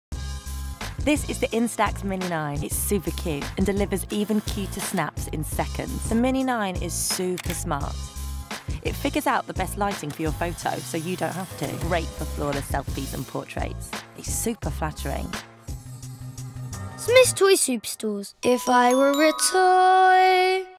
Fuji Instax Mini 9 Camera - Smyths Toys
It’s you, yourself & your mini 9, Meet your new best friend – the Instax mini 9. Cute, colourful and compact, this instant camera is just bursting with cool photo features and will always be by your side.